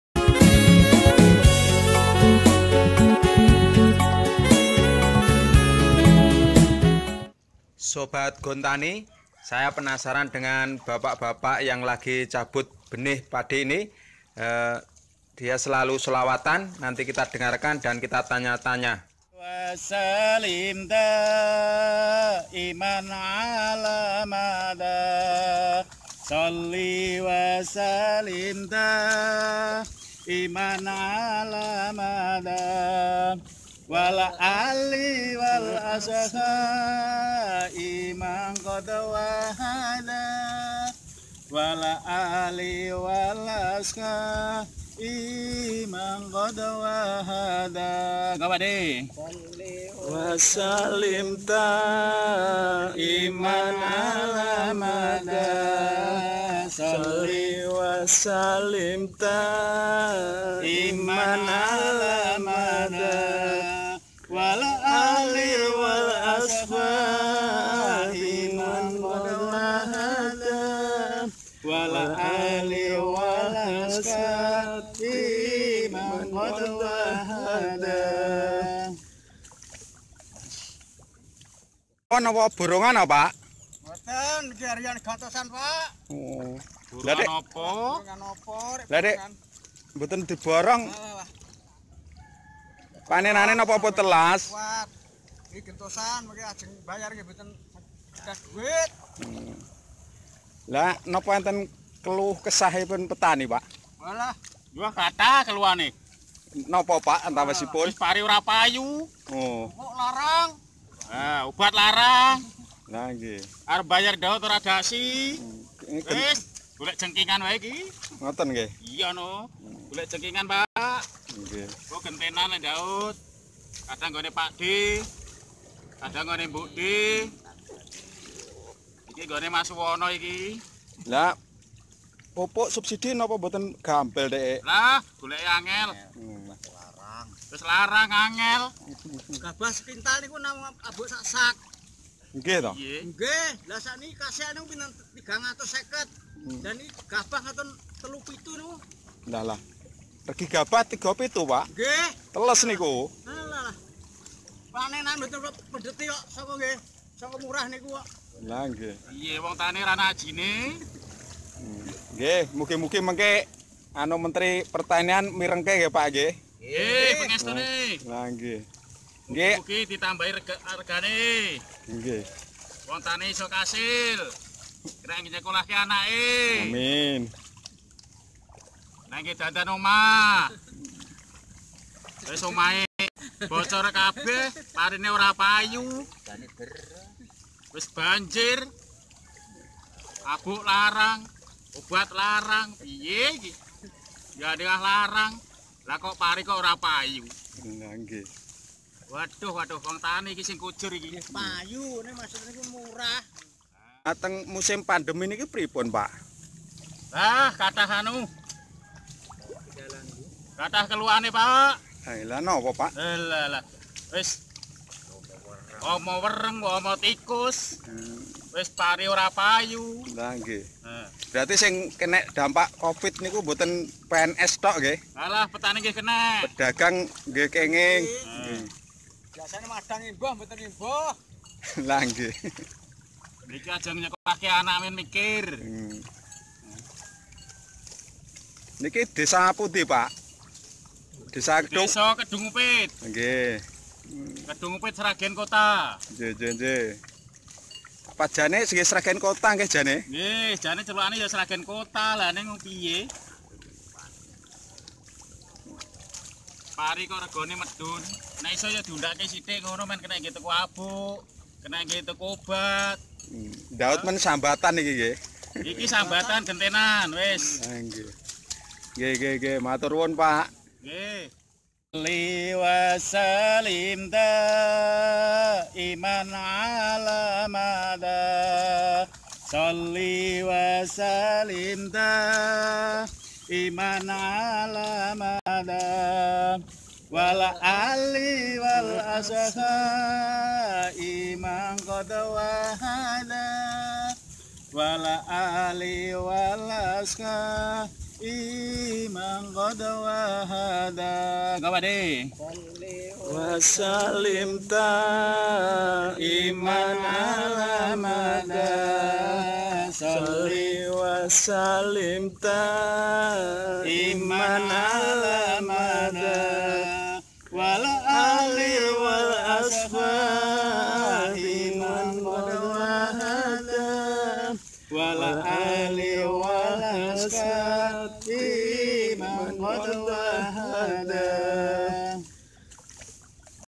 Petani Angkat Bicara
Video ini merupakan ungkapan isi hati para petani padi, mayoritas petani keluhkesahnya sama.